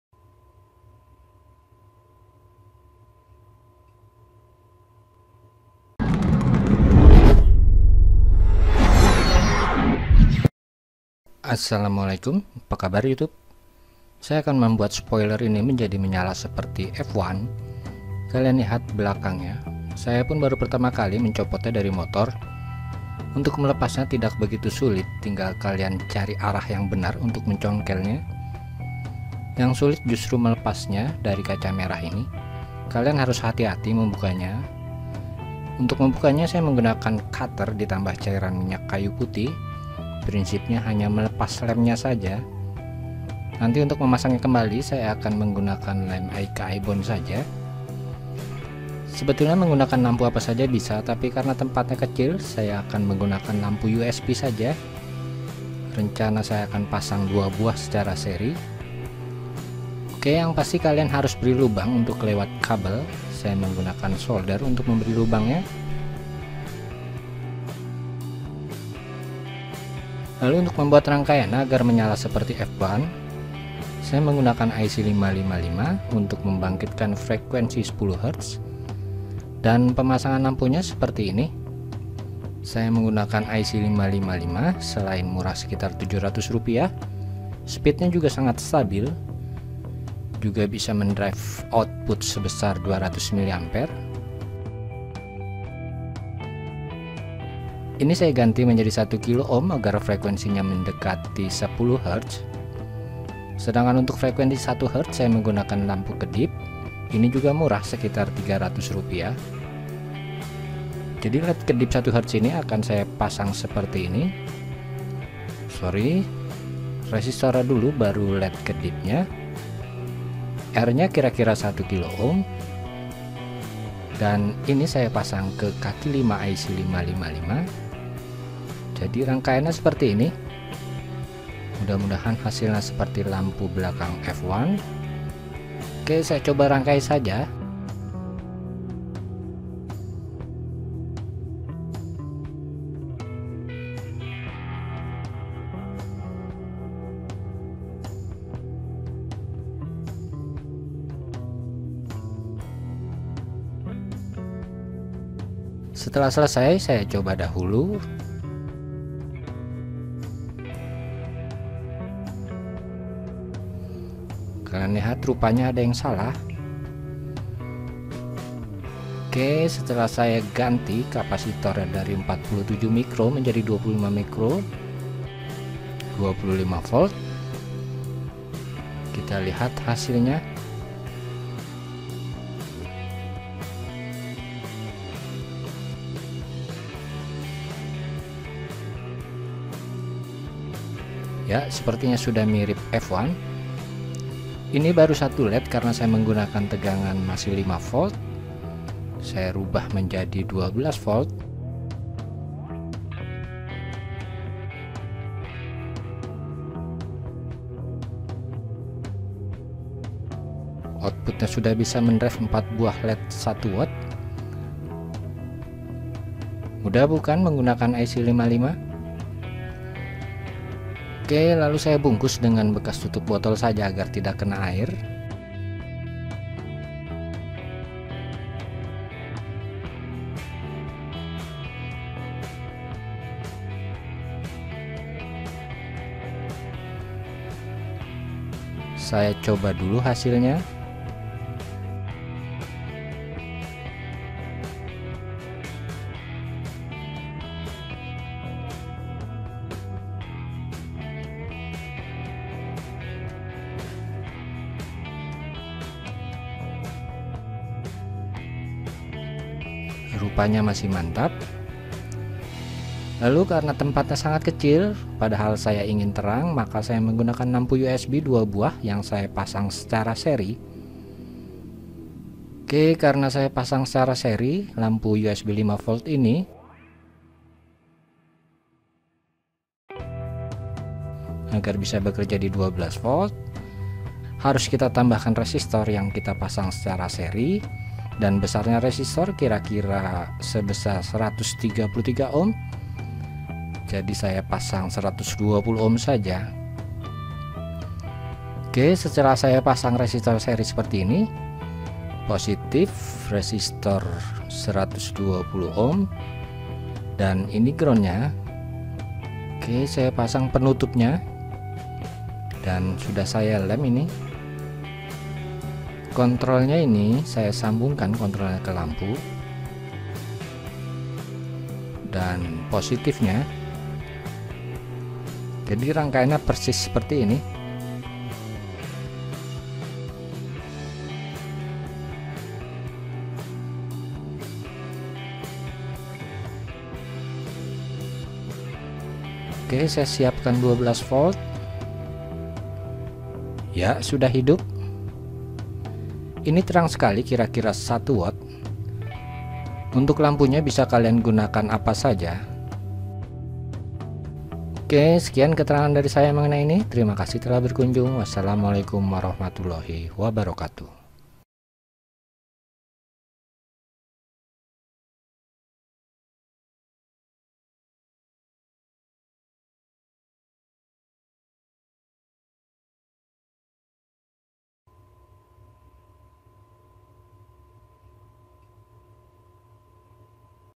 Cara Membuat Spoiler Menyala saat ngeRem ala Jetbus ala F1
Cara Membuat Spoiler Menyala saat nge-Rem ala Jetbus

00:00 Radal05
00:13 Spoiler F1
01:12 Skematik